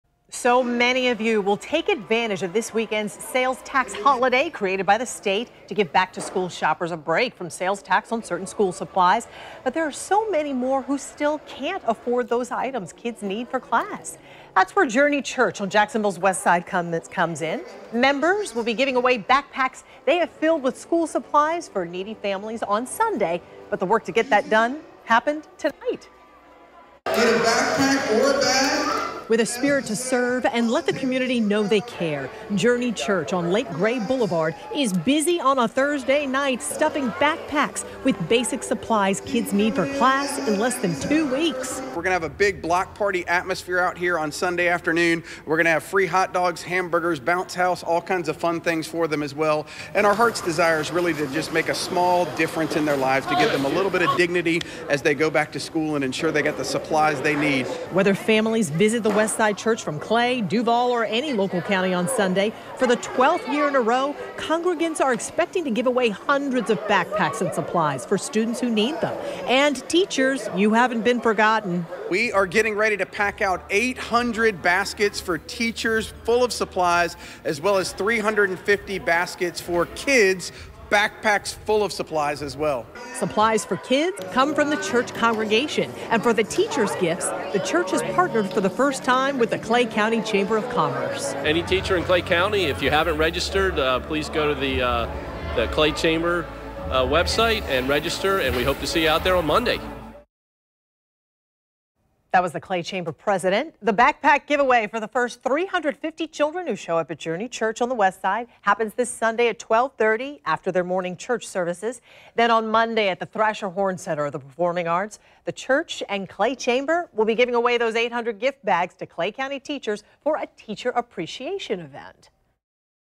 Members from Journey Church stuff backpacks for back to school event
Congregates are expecting to give away hundreds of backpacks and supplies